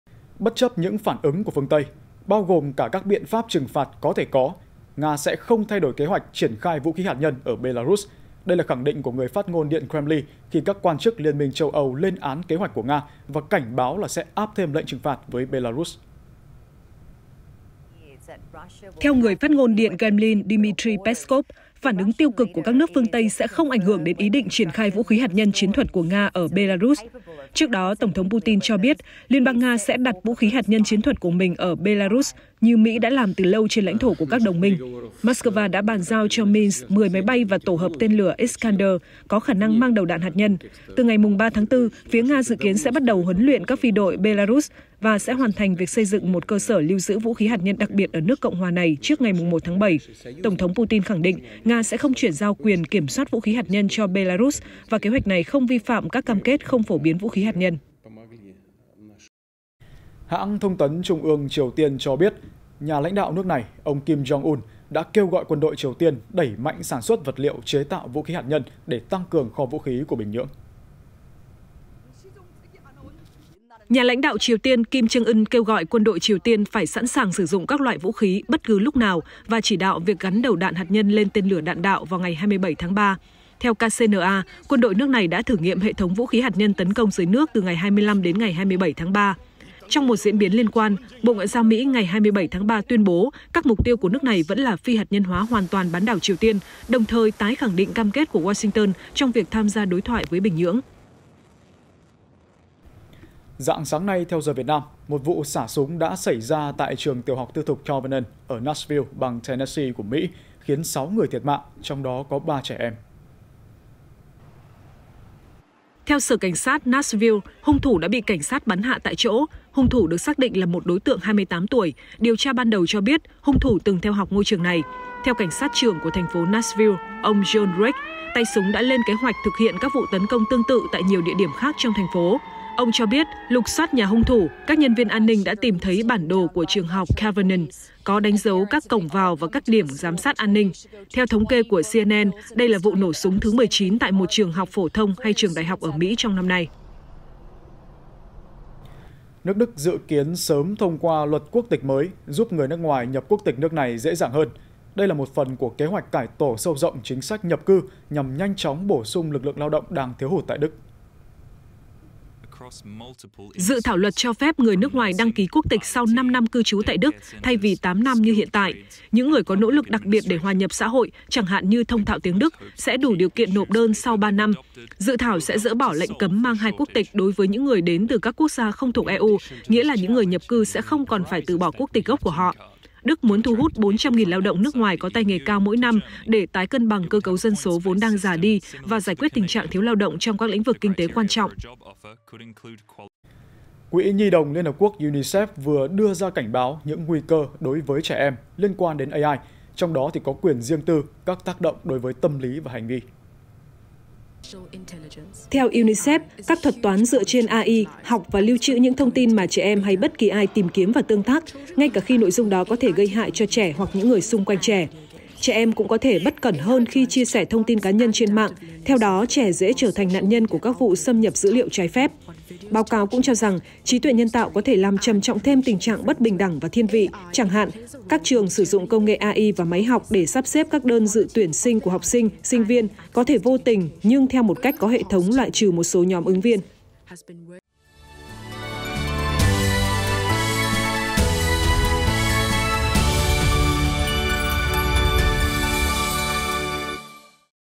Nga sẽ triển khai vũ khí hạt nhân ở Belarus
Nga sẽ triển khai vũ khí hạt nhân ở Belarus | Truyền Hình Nhân Dân
1. Dự Báo Thời Tiết Hôm Nay